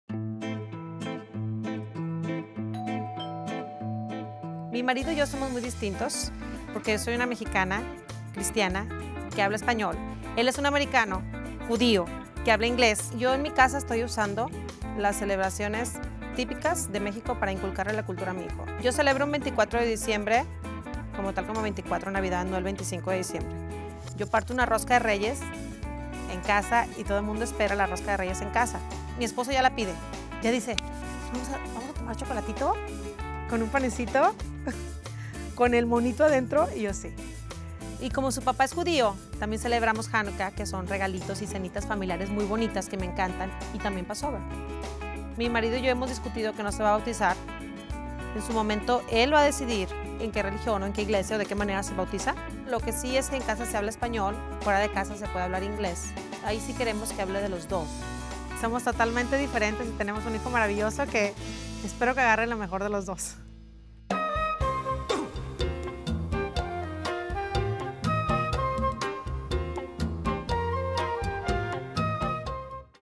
Gladis Fischler
Una familia, dos culturas... ¿qué le enseñas a tus hijos? Ve a Gladis compartir sus deseos para su niño. ¡Disfruta de sus aventuras y cuéntanos las tuyas